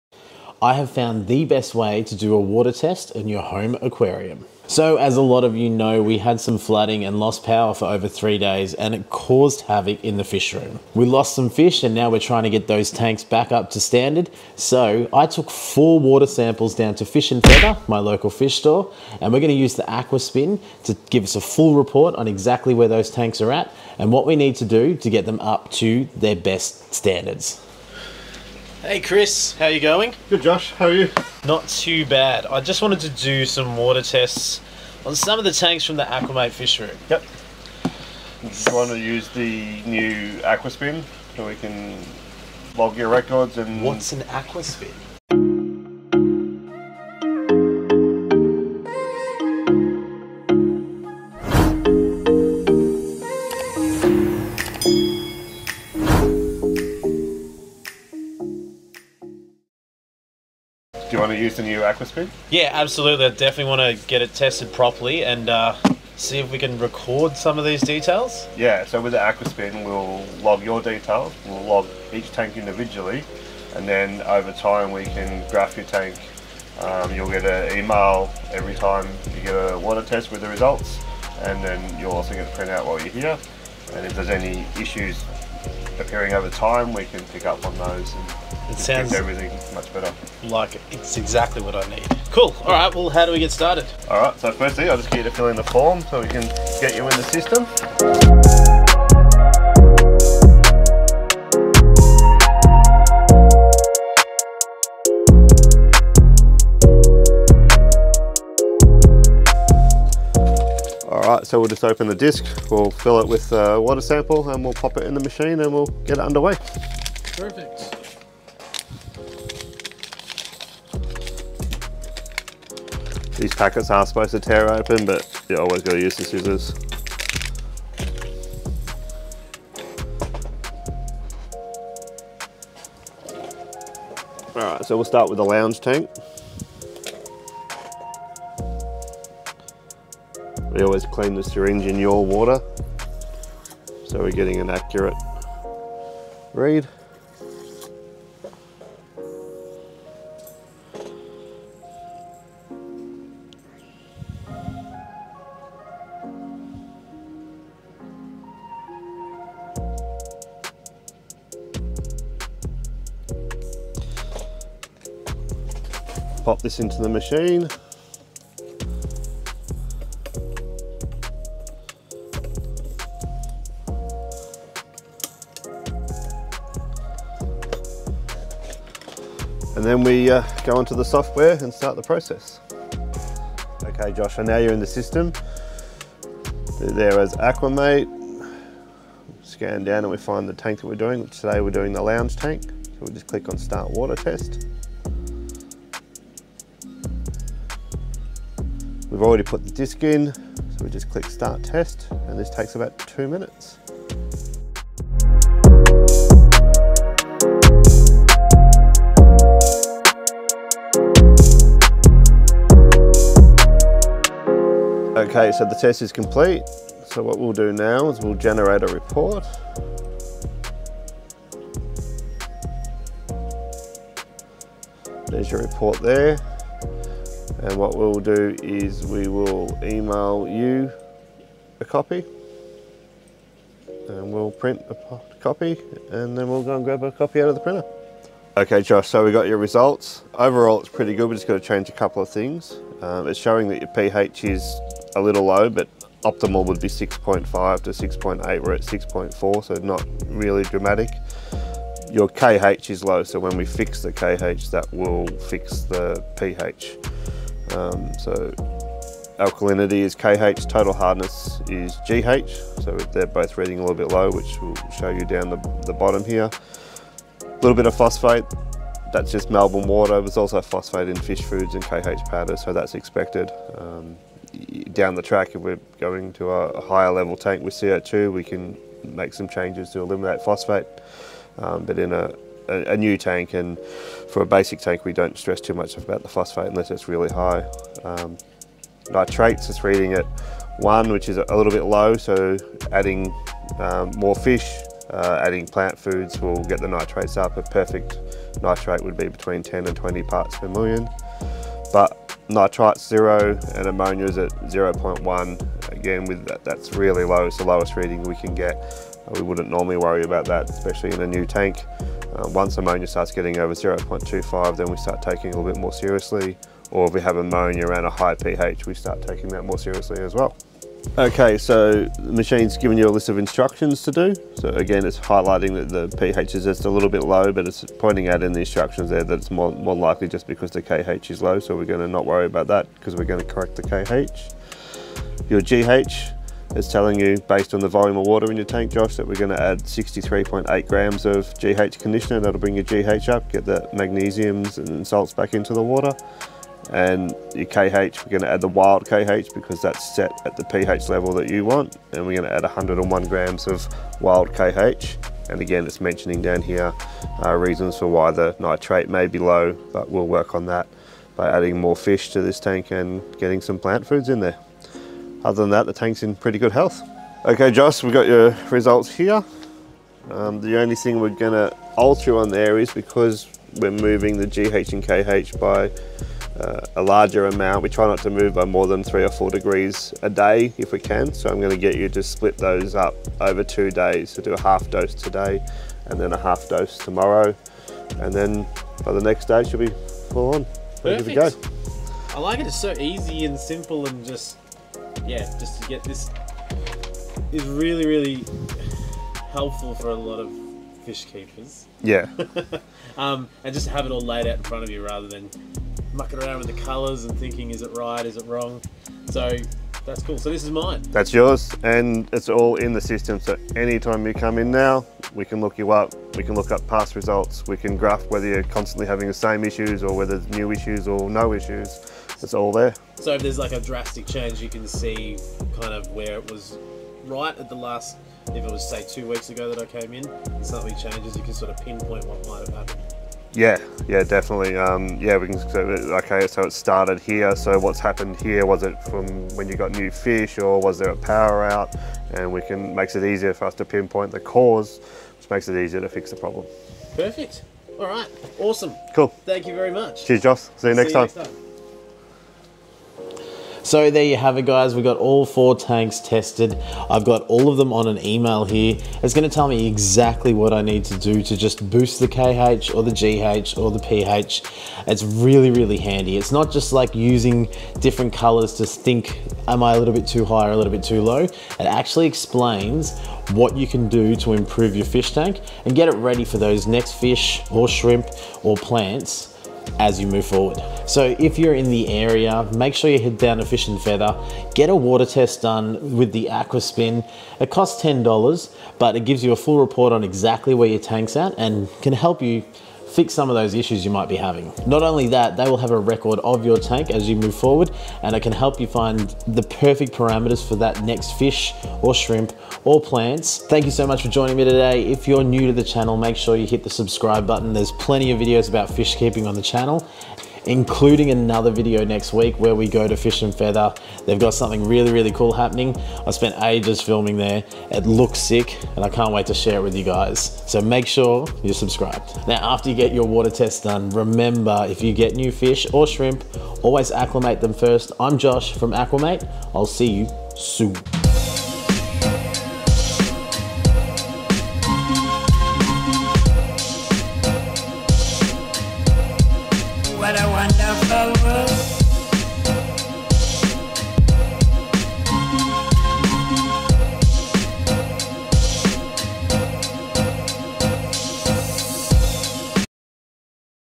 The Key to PERFECT Water Paramaters, The Aqua Spin water test at Fish and Feather
So I was lucky enough to use the future of water testing technology with Chris at Fish and Feather in Kilsyth.

This is the most comprehensive way to test your fish tanks water, fresh or salt!

And luckily for you all its available to anyone at Fish and Feather in Kilsyth.
So if you want to make sure your tank is in tip top condition, or fix some of those ongoing issues your having but cant pin point, Get down to Fish and Feather and get your water tested properly.